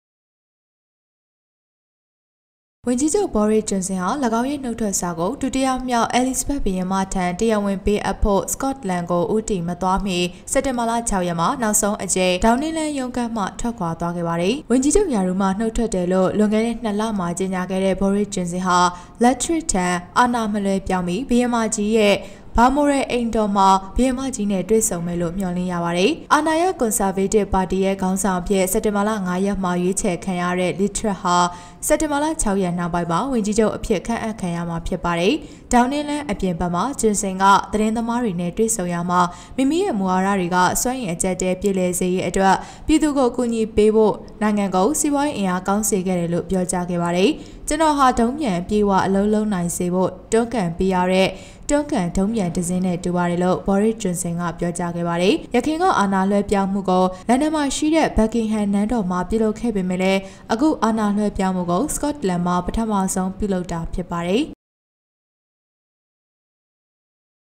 တိုင်းပြည်စီးပွားရေး ကောင်းမွန်စေရန် လုပ်ဆောင်ပေးခဲ့သည်ဟု ဘောရစ်ဂျွန်ဆင်ပြောကြား
တိုင်းပြည်စီးပွားရေး ကောင်းမွန်စေရန် လုပ်ဆောင်ပေးခဲ့သည်ဟု ဘောရစ်ဂျွန်ဆင်ပြောကြား (ရုပ်သံ)

ဝန်ကြီးချုပ် ဘောရစ်ဂျွန်ဆင်က သူ့ရဲ့ နုတ်ထွက်လွှာကို ဒုတိယမြောက် အဲလိစဘတ် ဘုရင်မထံ တရားဝင်ပေးအပ်ဖို့ စကော့တလန်ကို မသွားခင် စက်တင်ဘာ ၆ ရက်နေ့က ဒေါင်းနင်းလမ်းရုံးခန်းမှ ထွက်ခွာခဲ့ပြီး တိုင်းပြည်စီးပွားရေးစီးပွားရေး ကောင်းမွန်စေဖို့ လုပ်ပေးခဲ့တယ်လို့ ပြောကြားခဲ့ပါတယ်။